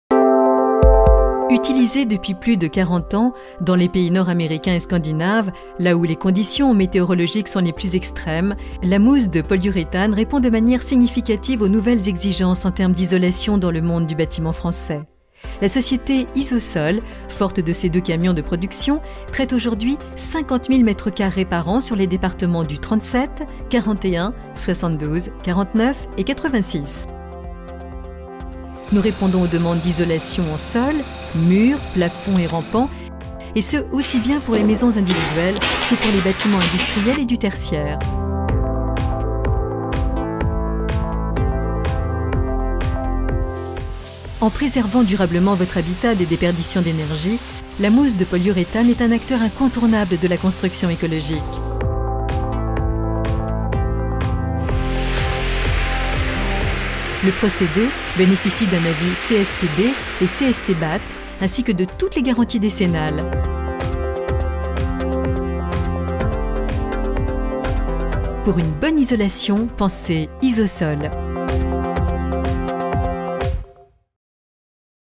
ISOSSOL à Monts, isolation par mousse de polyuréthane.
L'entreprise ISOSSOL est installée à Monts dans le département de l'Indre-et-Loire. Elle se déplace dans toute la région Centre ainsi que les départements limitrophes pour tous travaux d'isolation. Spécialistes de l'isolation par mousse de polyuréthane, nous vous assurons une excellente performance et durabilité des matériaux pour l'isolation des toits, des murs par l'intérieur et l'extérieur, ainsi que l'isolation des sols.
Nos produits sont par ailleurs conformes à la norme RT 2012 et aux maisons BBC.